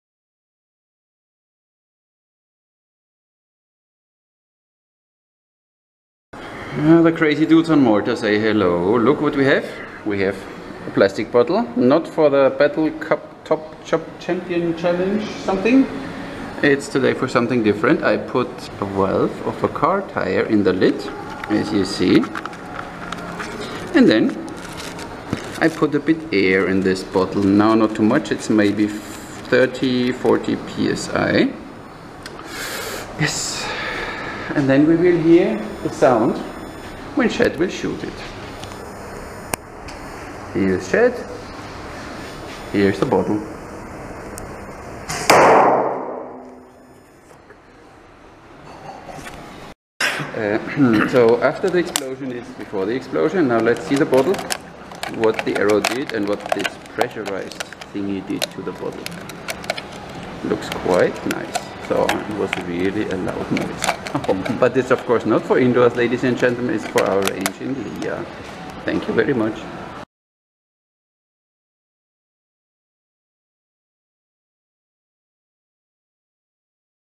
Shoot the (pressurized) bottle with an arrow
Experimenting with pressurized plastic bottles. A car tire valve in the cap and pumped up. Definitely not for indoors :D